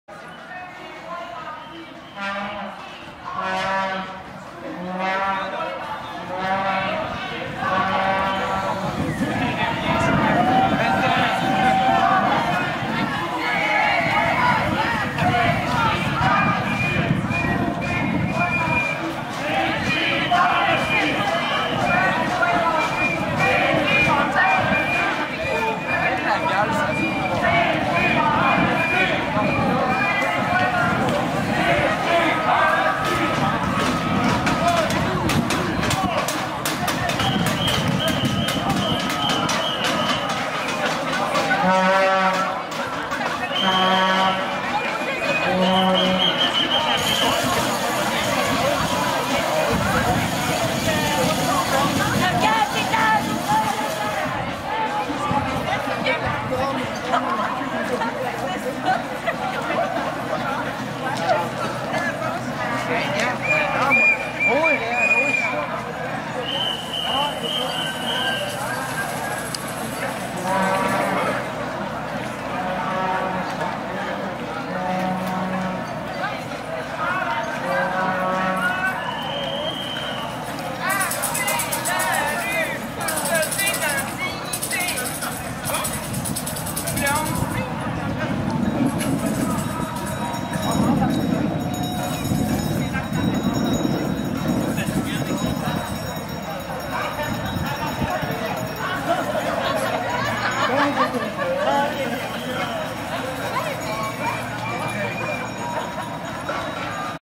La Belle Province? Manifestation contre les coupures à l'aide sociale jeudi 11 avril 2013 (Clip 6)
Manifestation contre les coupures à l'aide sociale jeudi 11 avril 2013 (Clip 6)